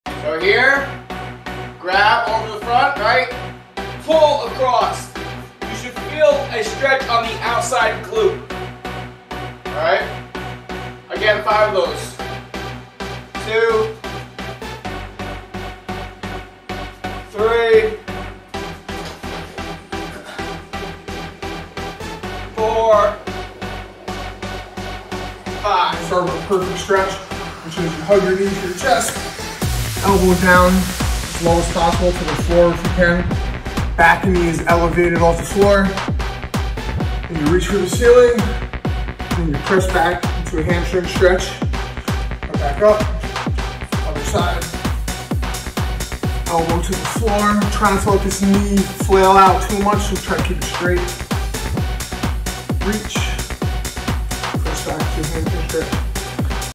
MMA Style Workout Warmups | Dennis Bermudez | Jared Gordon
MMA workout warmups with UFC fighters Jared "Flash" Gordon and Dennis "Menace" Bermudez. Watch how UFC fighters get warmed up for their workout routines to avoid injury and get in shape to fight in the Octagon.